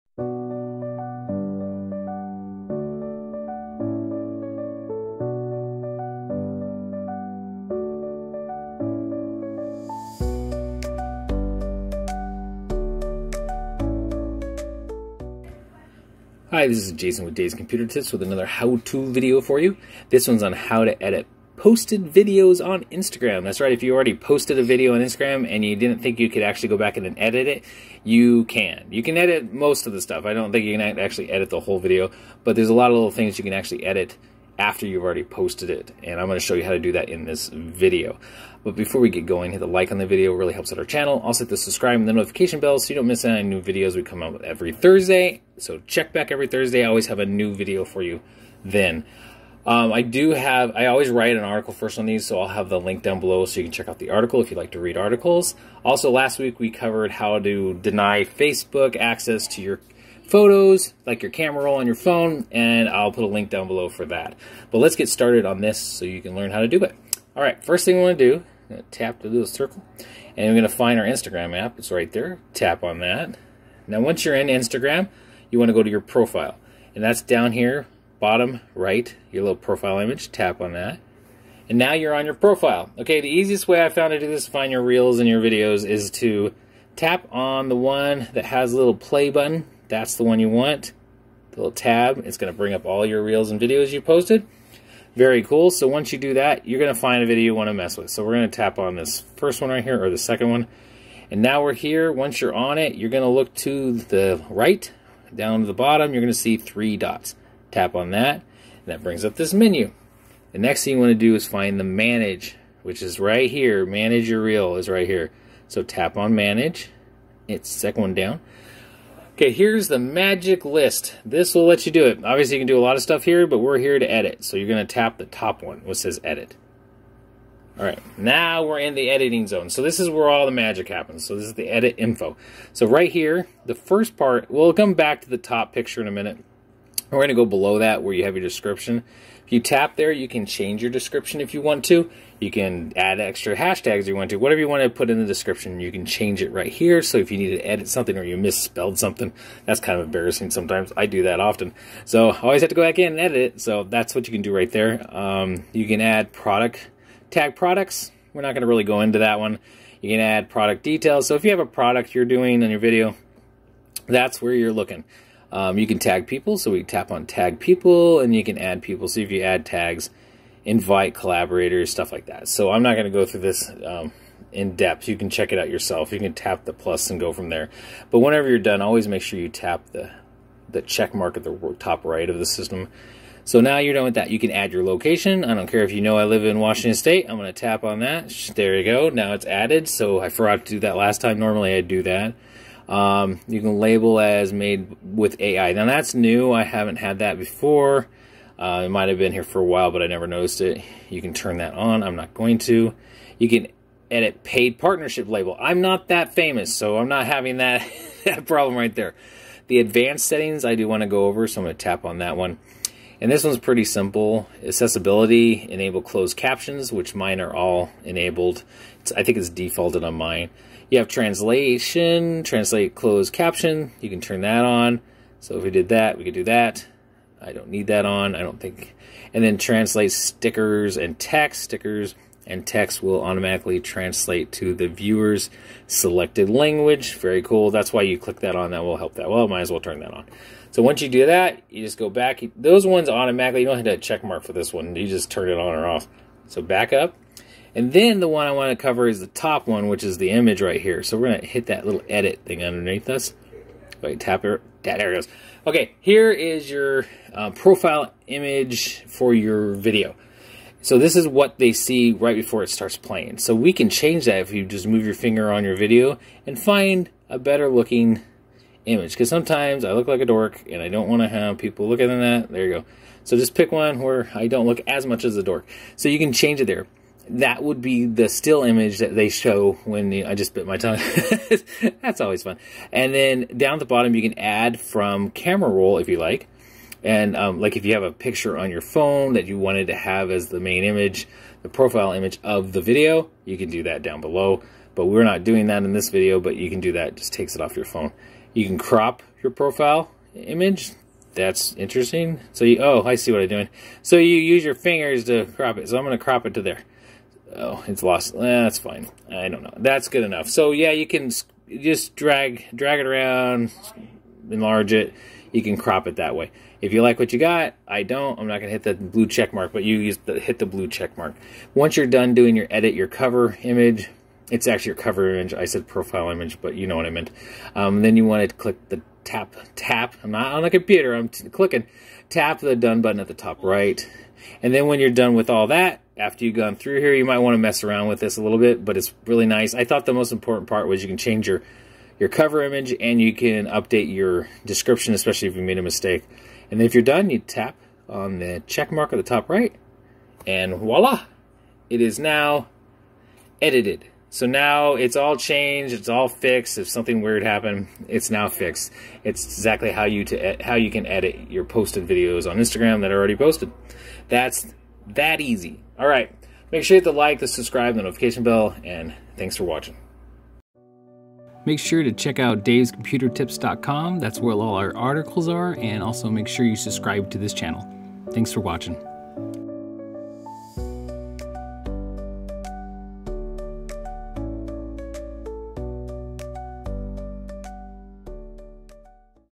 How To Edit Posted Videos On Instagram
In this video I will show you how to edit video reels you already post.

Last week, we covered how you could deny Facebook access to your camera roll. If you missed that, you can find it here:

This week, I’d like to switch to another social media platform. What is this other app you speak of, you might be asking? That would be Instagram, and I wanted to show you how you can edit a video that you already posted. You might not have known that after you post something, you can still edit the post. I will be focusing on video (Reel) posts that I have done. Before I start our lesson, make sure to check out these other videos I have created on Instagram:

Now that that is out of the way, let us start our class.